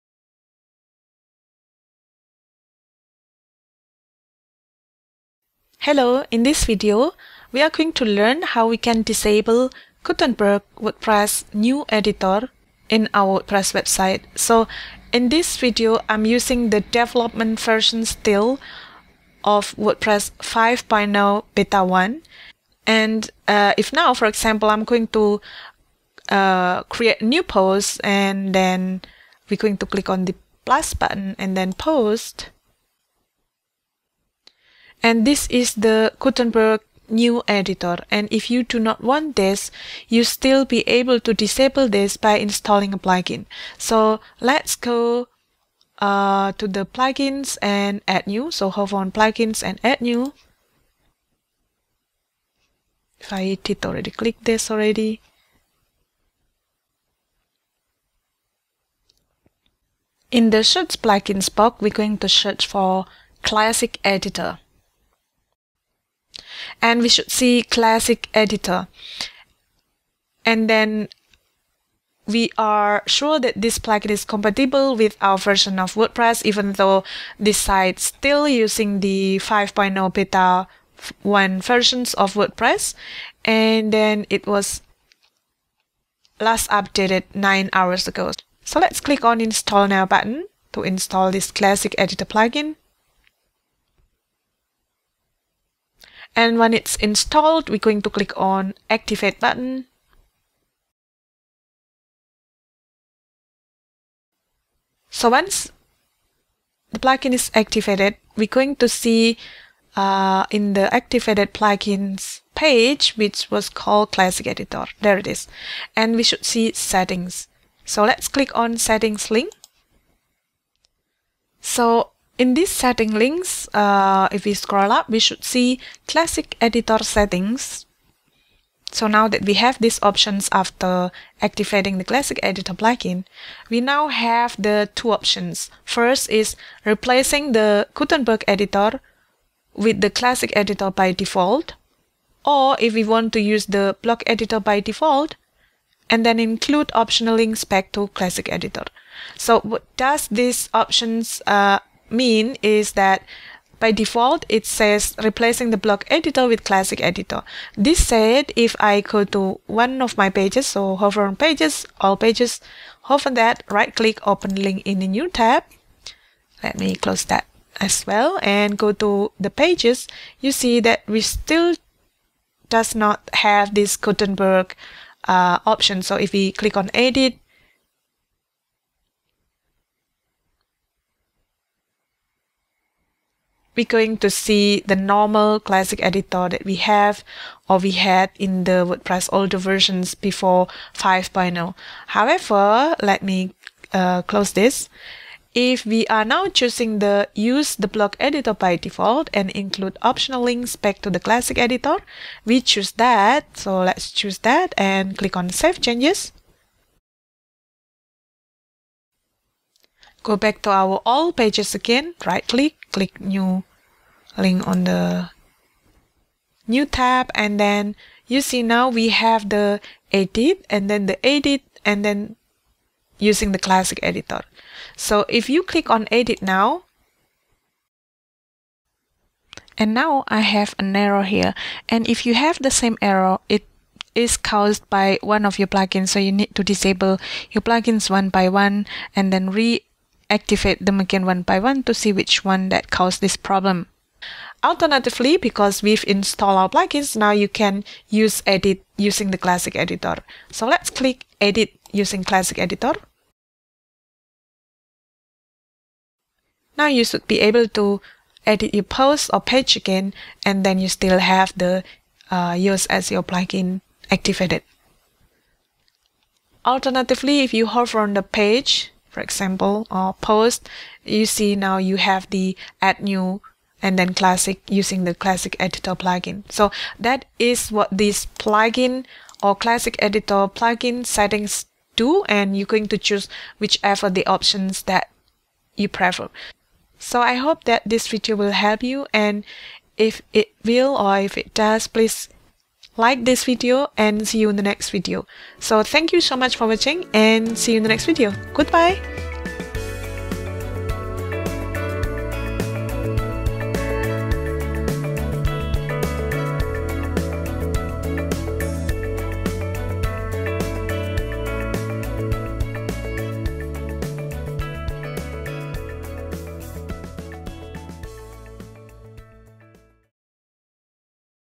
How To Disable Gutenberg Editor WordPress 5.0 Using a Plugin
In this video, we will learn how to disable the Gutenberg Editor that will be shipped with WordPress 5.0 using Classic Editor Plugin.

This video uses WordPress 5.0 Beta 1 version, but the functionality of the Classic Editor plugin will likely to be the same.

0:45 Installing "Classic Editor Plugin". Or you can download it from WordPress repository

1:58 Classic Editor Plugin settings

3:50 An error that you may have